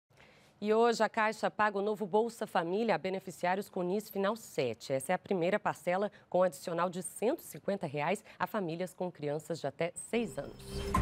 ECONOMIA | Pagamento Bolsa Família
A Caixa paga nesta terça-feira (28), o novo Bolsa Família a beneficiários com NIS final 7. Essa é a primeira parcela com adicional de R$ 150 a famílias com crianças de até 6 anos.